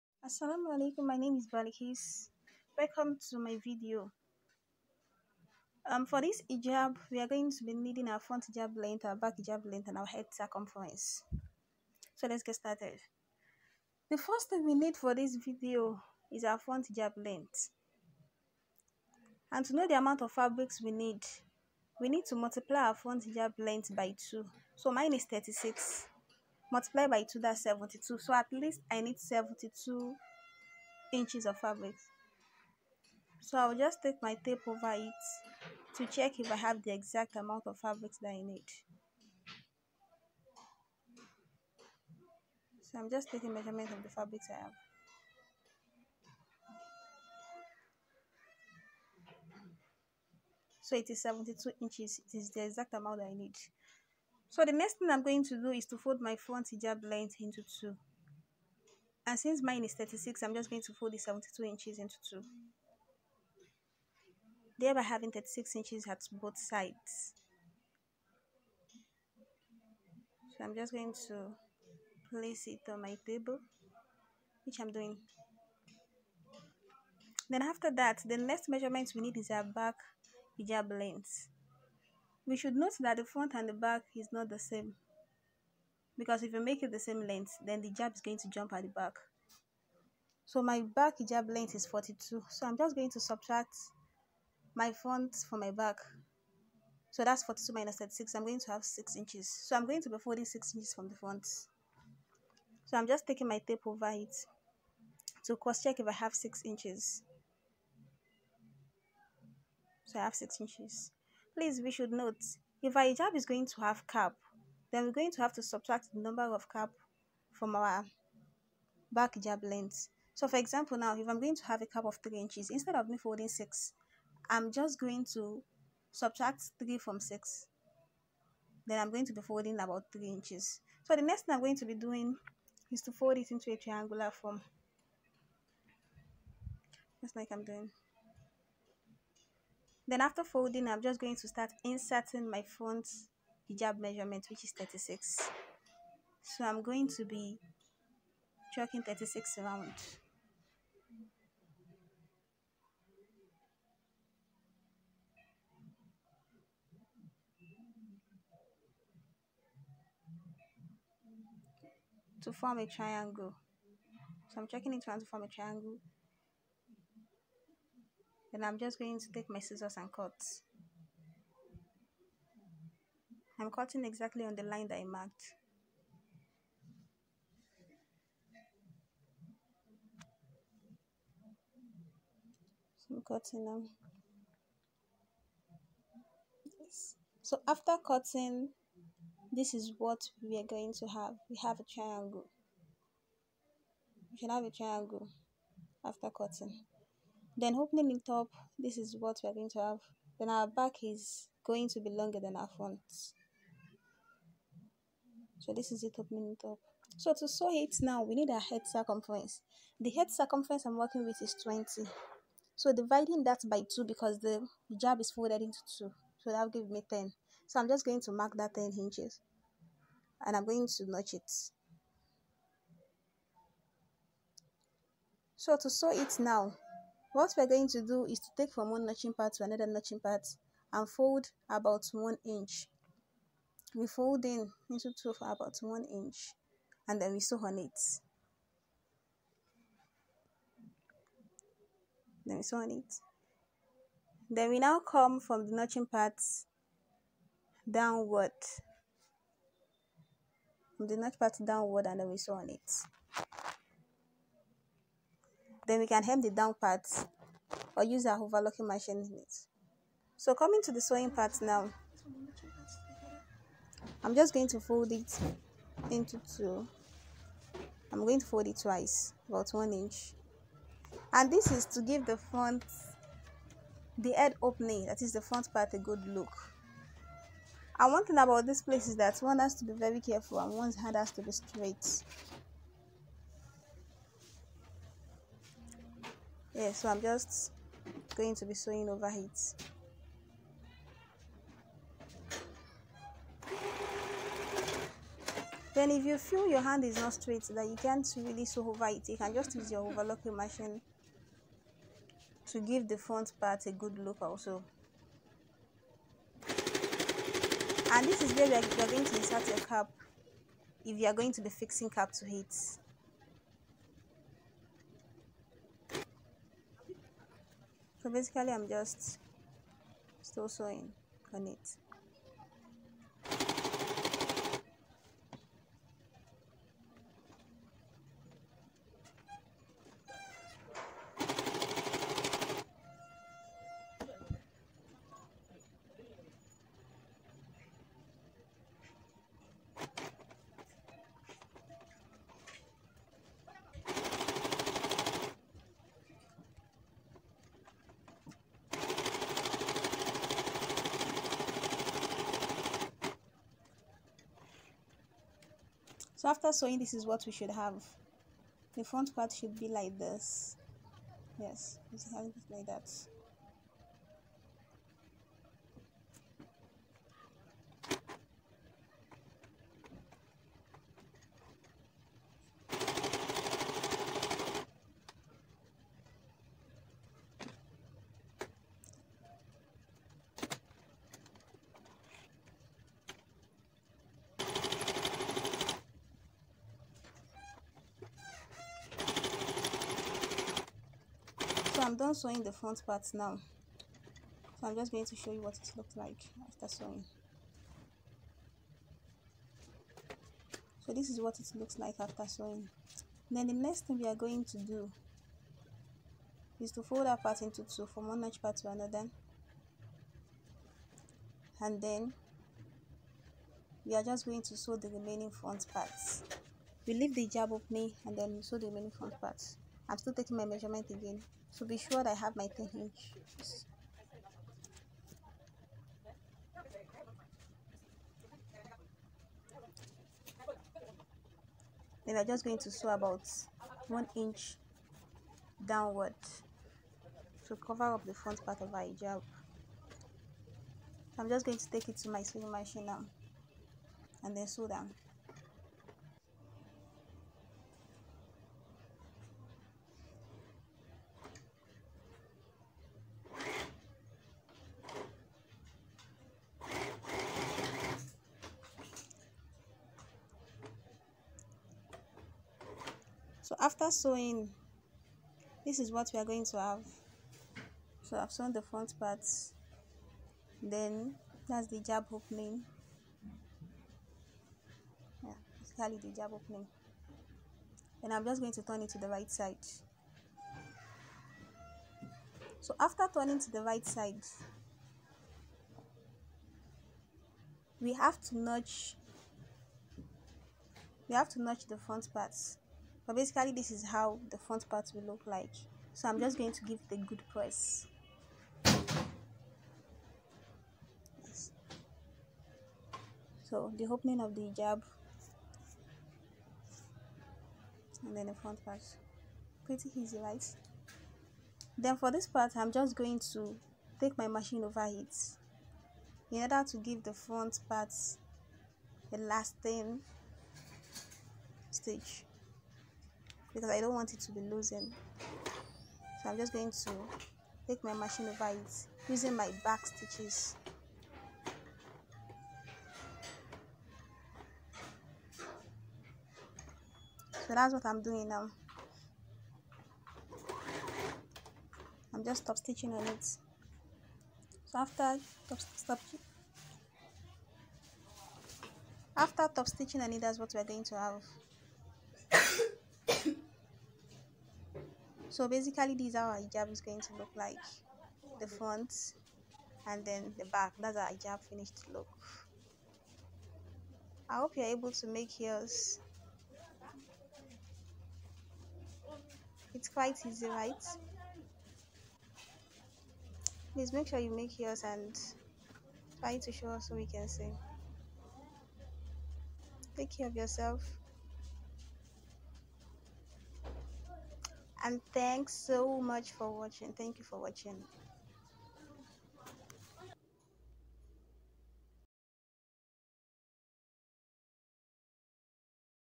DIY How to cut and sew hijab(very easy)| My first youtube video😚
Materials needed
2yards of fabrics
taperule
scissors
chalk

pls click these links for more hijab DIY